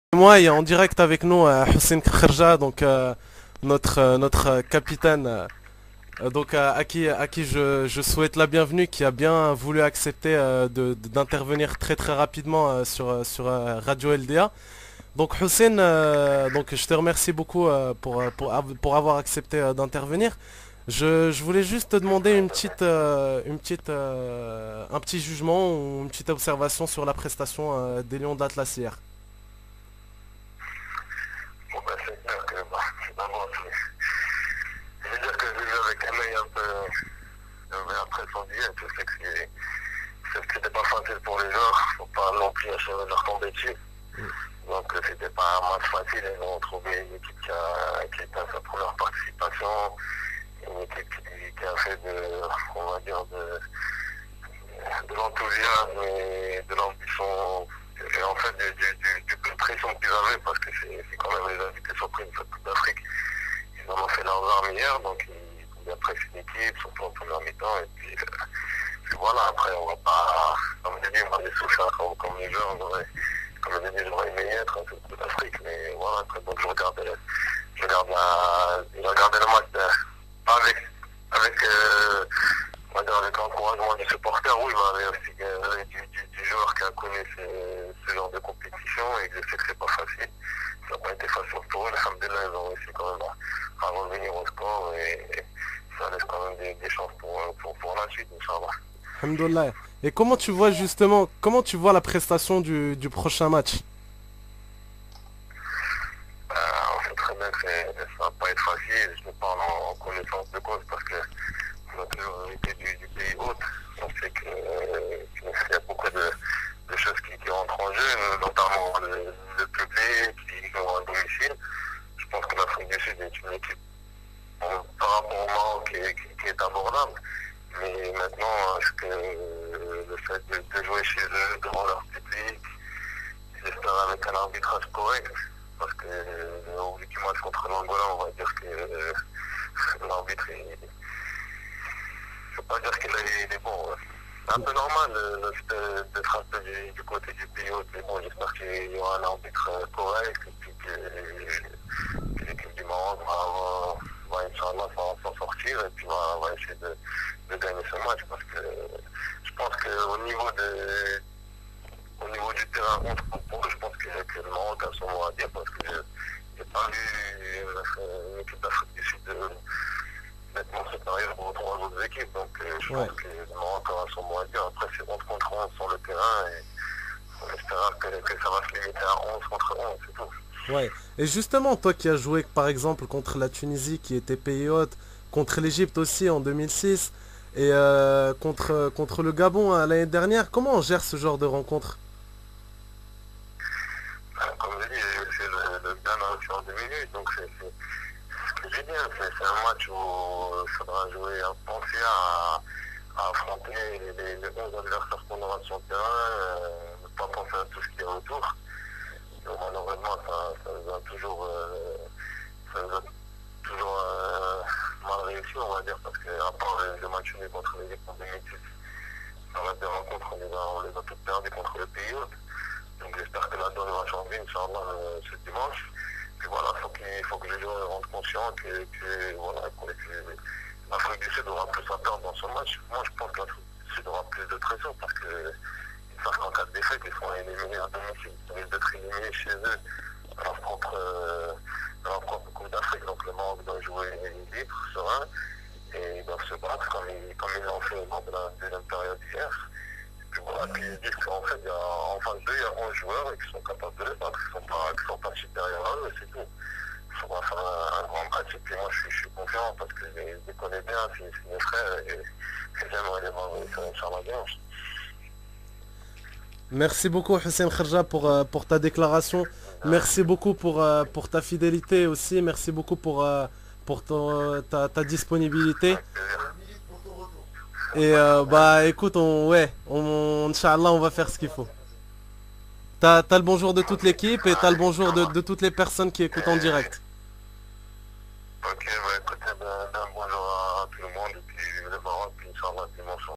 Exclusivité : Houcine Kharja sur RadioLDA
Réaction de Houcine Kharja sur la RadioLDA après le match Maroc-Cap Vert